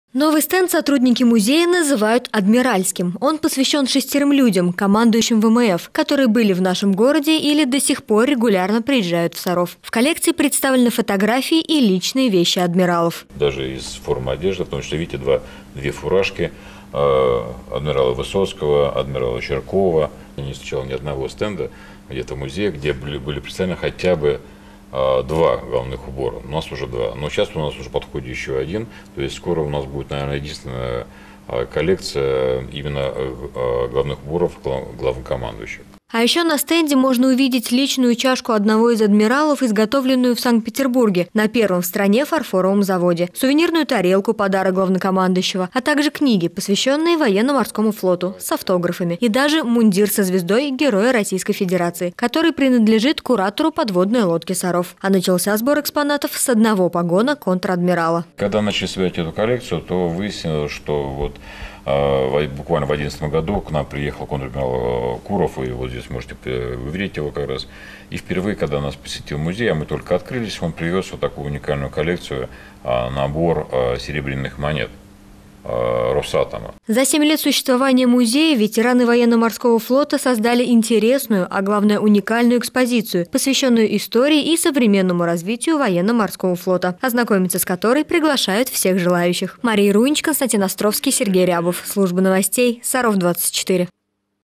Новая экспозиция в Музее ВМФ
Музей военно-морской славы существует в нашем городе уже 7 лет. В этом году здесь появились новые стенды.